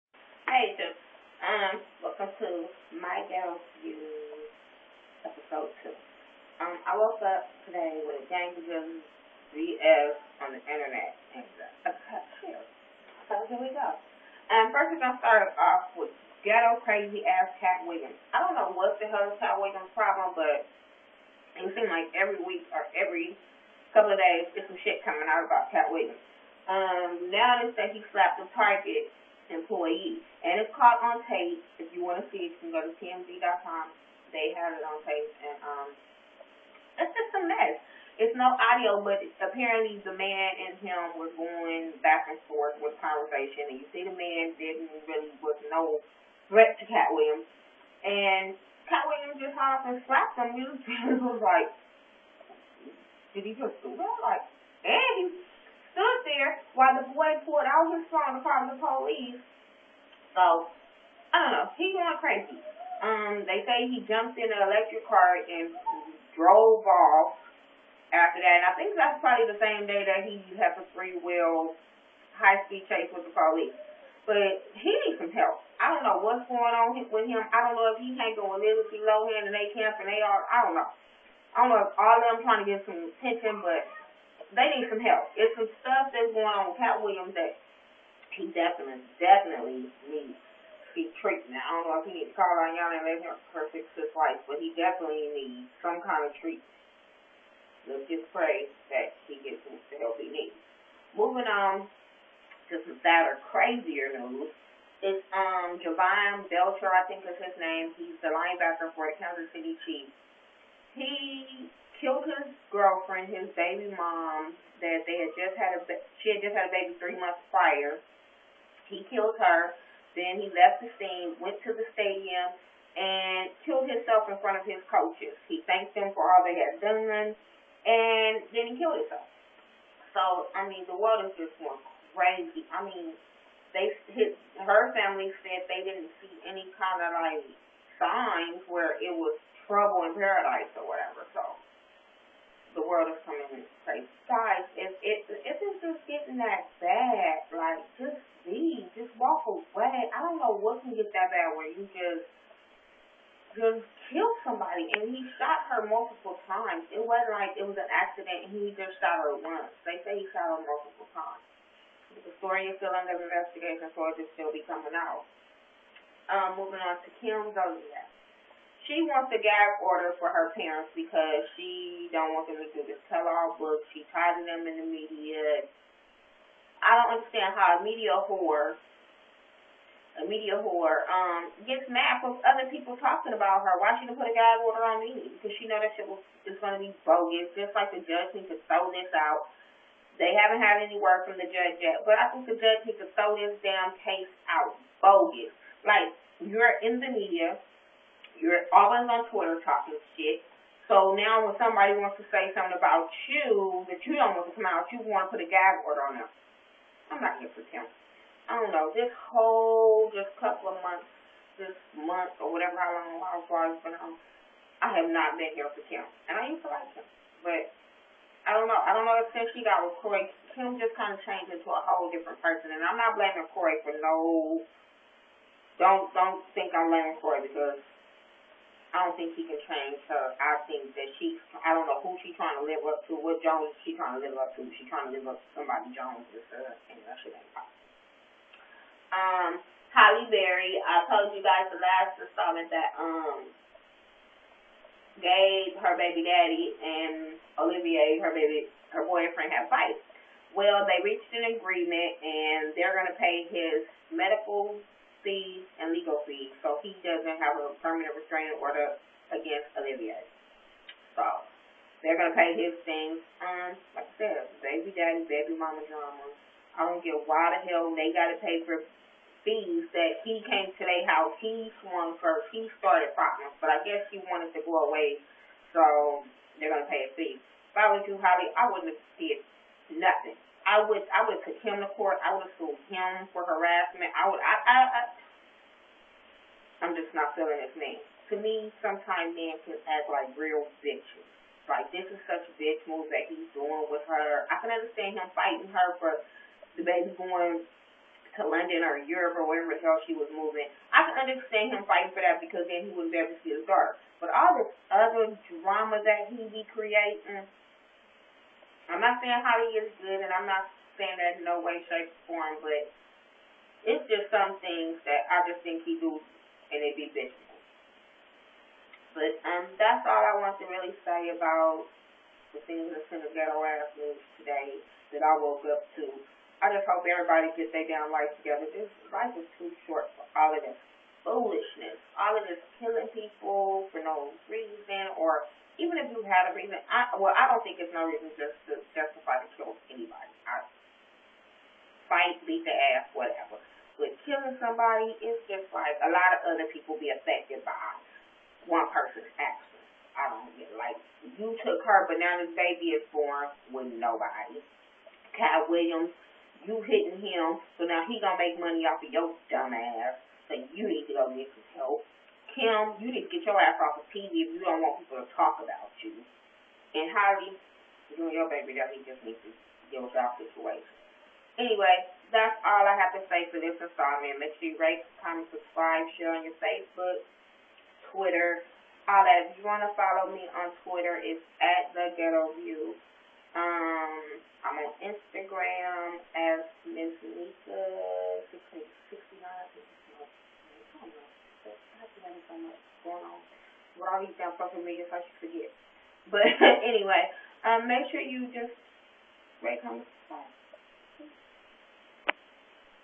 My Ghetto Views #2 (Katt Williams, Jovan Belch etc
My opinion of celebrities that are in the news.